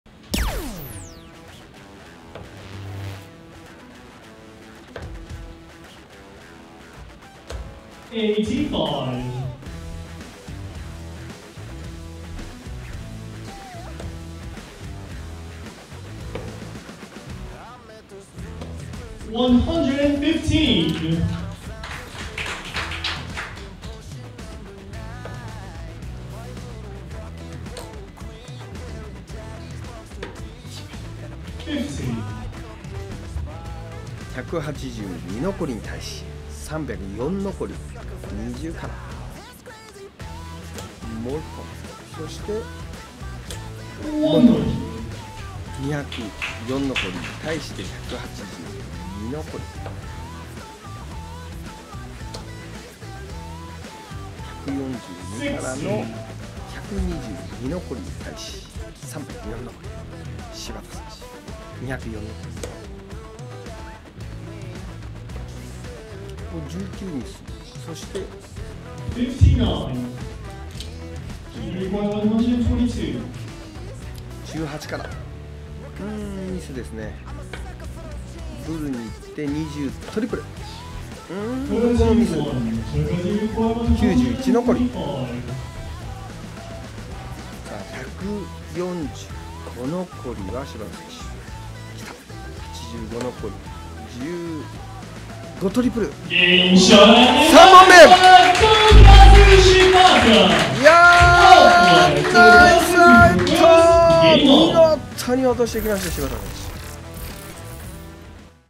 『145out』Shibata vs Matsuda【2021 PDC Asia World Cup Japan Qualifying】
2021.8.1
2021 PDC Asia World Cup Japan Qualifying

【決勝戦】
シバタ トヨカズ vs マツダ ジュン